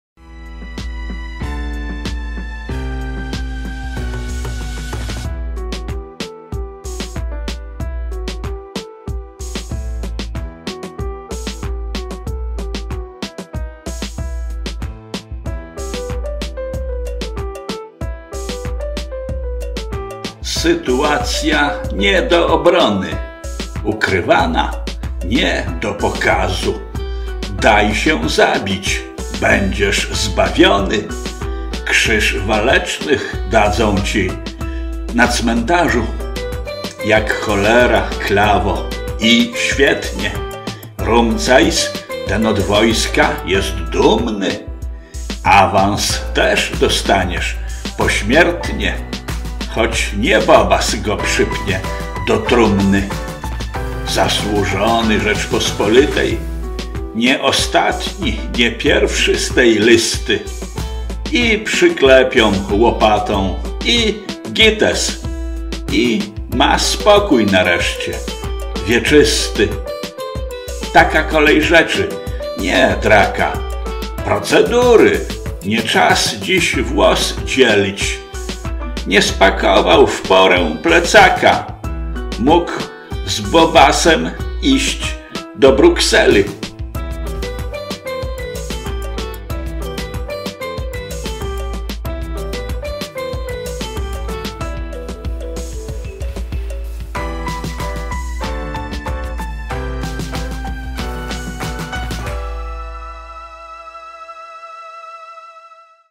Taka kolej rzeczy
Sytuacja nie do obrony.
Ukrywana. Nie do pokazu.
Daj się zabić - będziesz zbawiony.
Krzyż Walecznych dadzą ci.
Na cmentarzu.

Jak cholera klawo. I świetnie.
Rumcajs - ten od wojska - jest dumny.
Awans też dostaniesz. Pośmiertnie.
Choć nie Bobas go przypnie do trumny.

Zasłużony Rzeczpospolitej.
Nie ostatni, nie pierwszy z tej listy.
I przyklepią łopatą. I gites.
I ma spokój nareszcie.
Wieczysty.

Taka kolej rzeczy. Nie - draka.
Procedury. Nie czas dziś włos dzielić.
Nie spakował w porę plecaka.
Mógł z Bobasem iść do Brukseli.
6 czerwca 2024